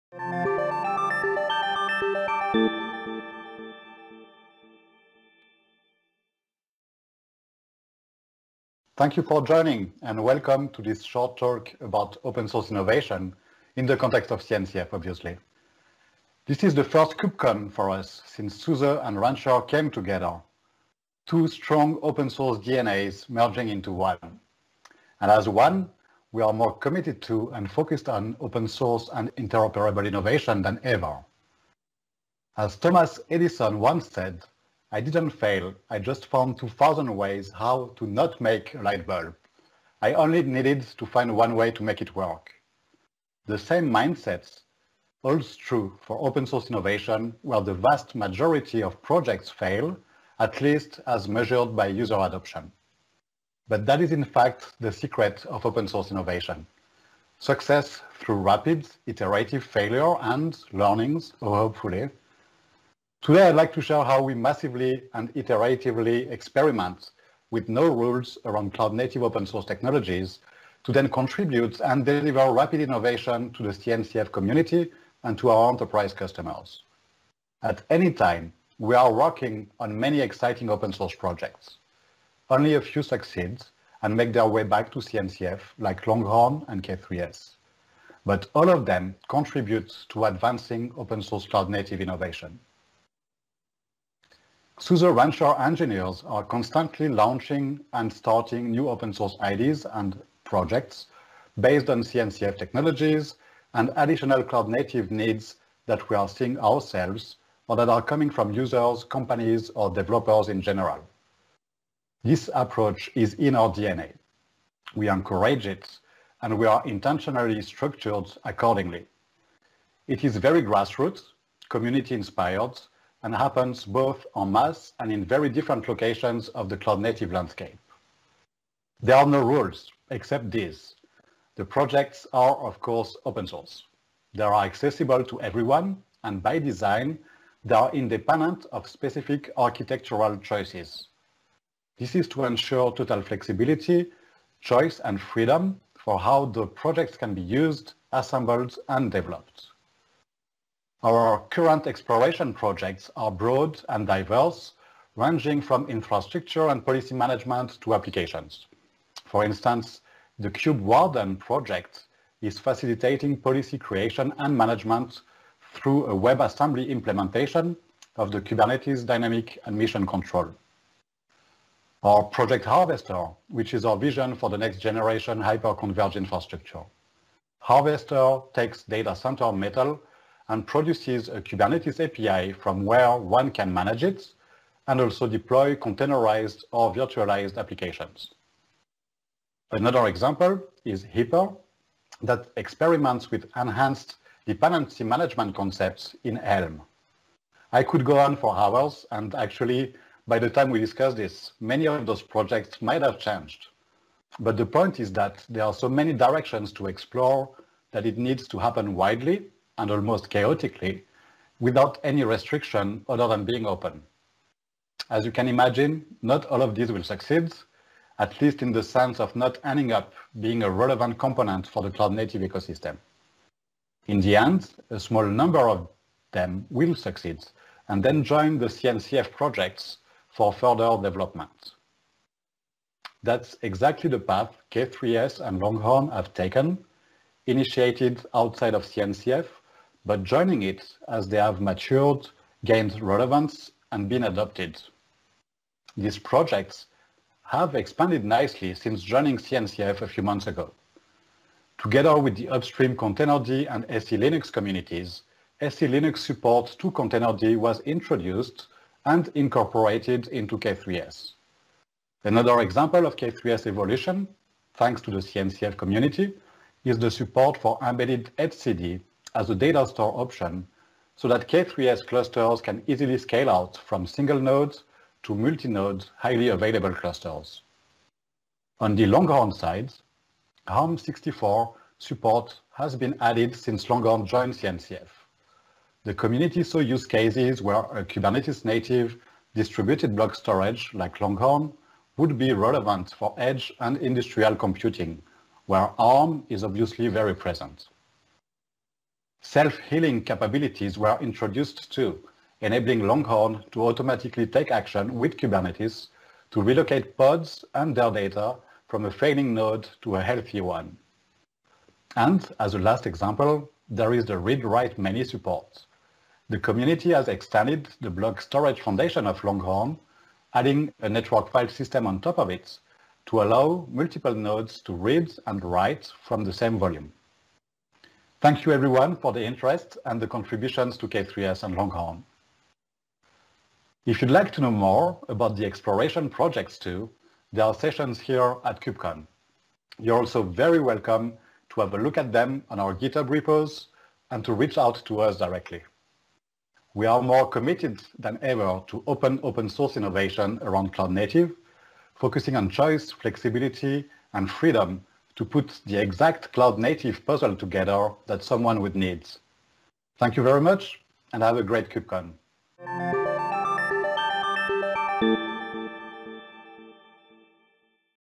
Sponsored Keynote: Open Source Innovation – Success Through Failure - Thomas Di Giacomo, SUSE
Sponsored Keynote: Open Source Innovation – Success Through Failure - Thomas Di Giacomo, Chief Technology & Product Officer, SUSE